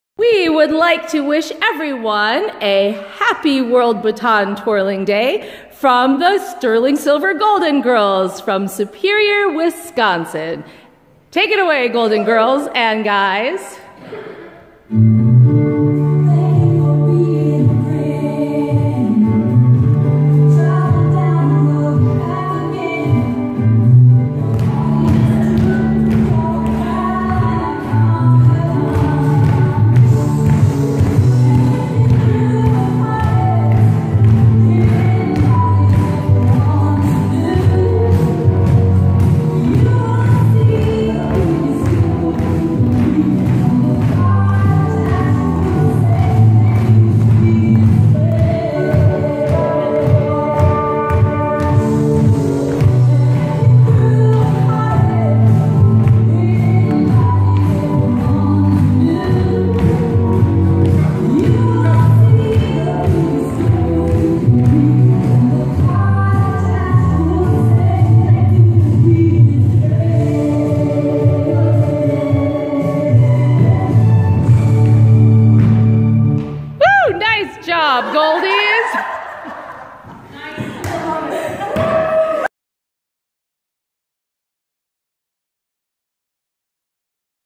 Sterling Silver Golden Girls & Guys Celebrating World Baton Twirling Day!
World Baton Twirling Day is April 10! Their class was on World Baton Twirling Day & wanted to celebrate the special day the Golden Girl way.